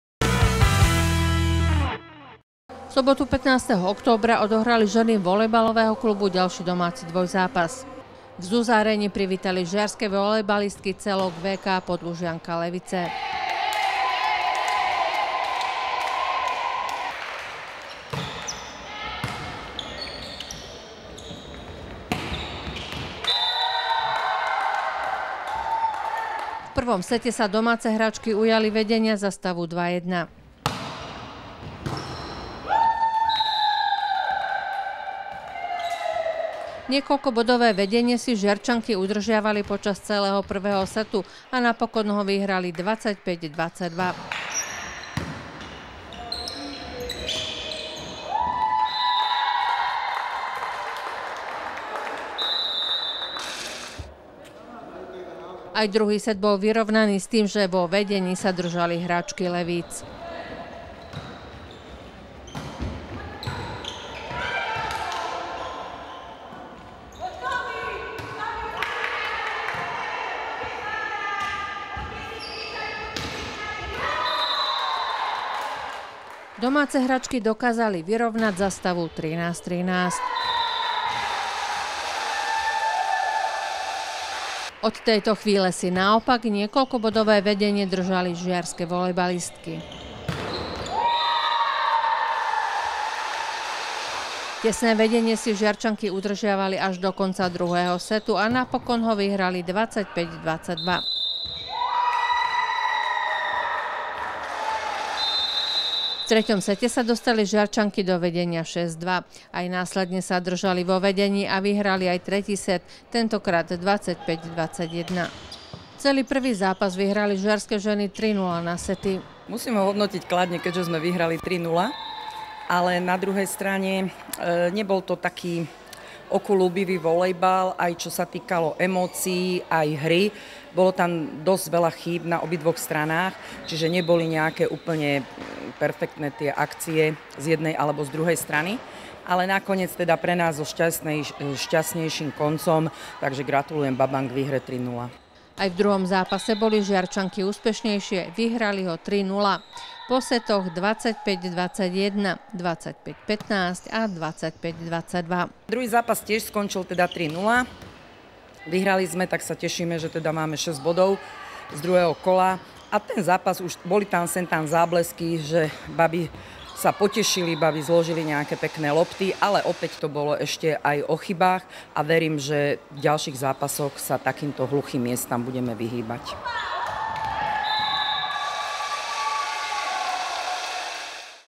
Žiarseé volejbalistky privírali VK Podlužianka Levice
V sobotu 15. októbra odohrali ženy ďalší domáci dvojzápas.